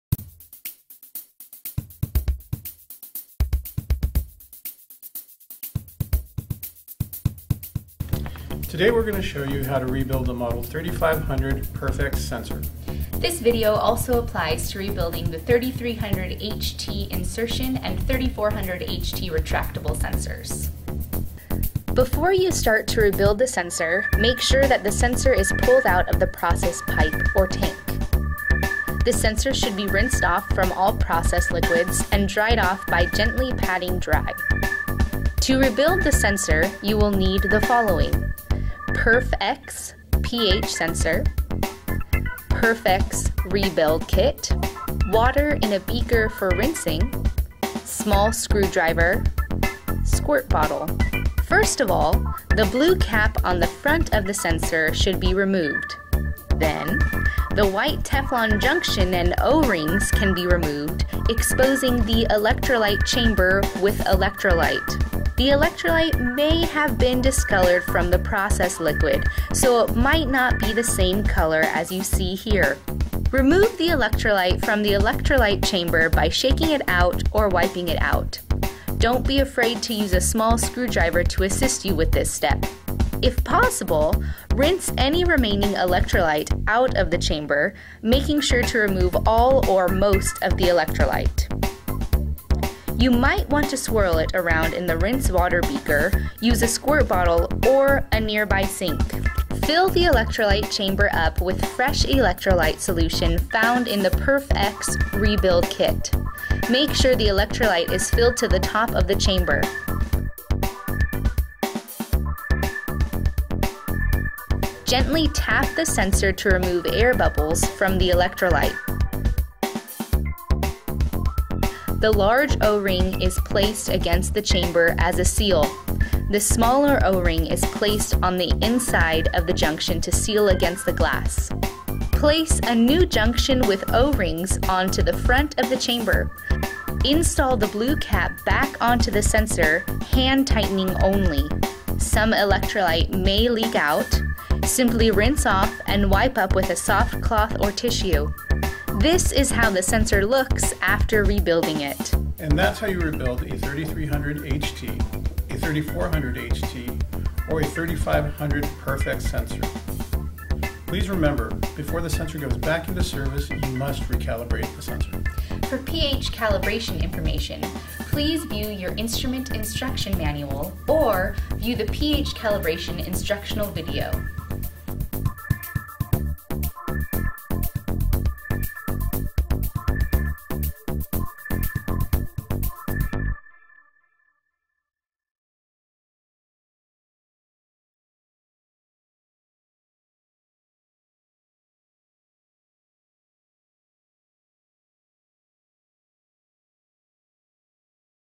PERpH-X: Sensor Rebuilding
The PERpH-X high performance pH sensors incorporate SMART technology and design innovations that prolong the life of the sensor in harsh applications. Watch this video to learn how to rebuild a 3300HT, 3400HT and 3500 PERpH-X™ sensors.